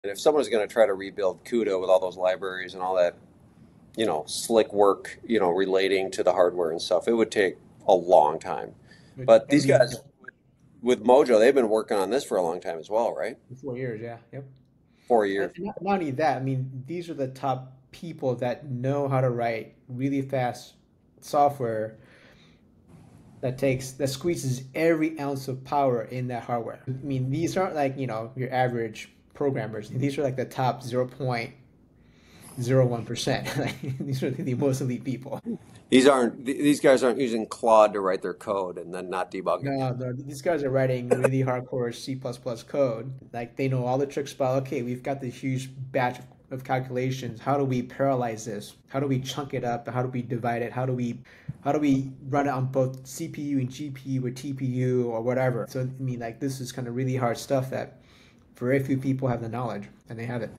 Why Mojo Devs Are Built Different 🔧🔥
Rebuilding CUDA from scratch? That’s child's play compared to what these Mojo devs are doing. We’re talking top 0.01% engineers writing elite C++ and squeezing every drop of power from hardware. No Claude, no fluff — just raw optimization. 💻🔥